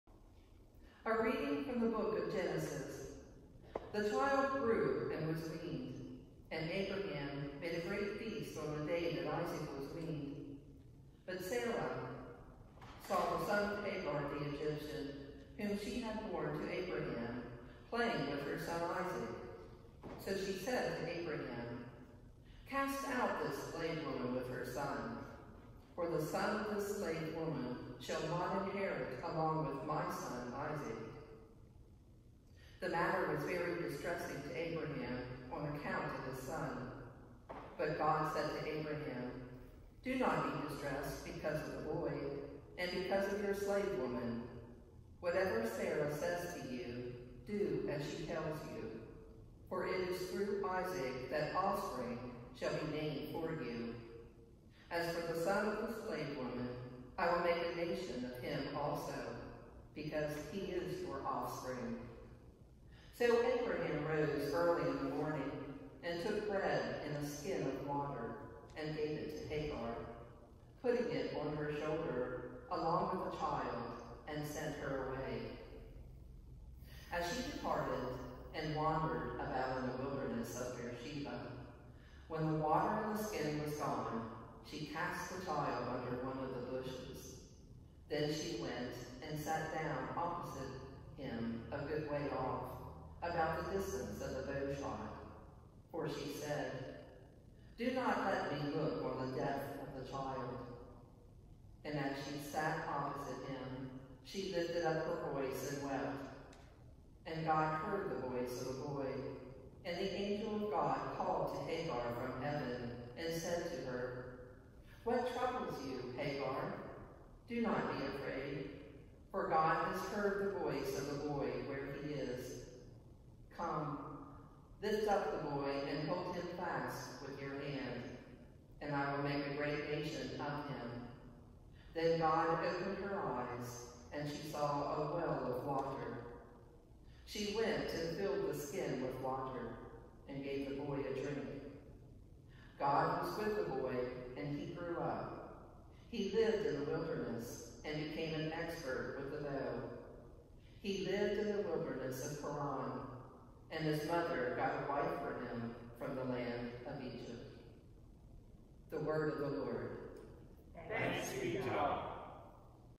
First Reading for the Third Sunday after Pentecost 2020
Genesis 21:8-21    Read by Jakki Rogers for Sunday, June 21, 2020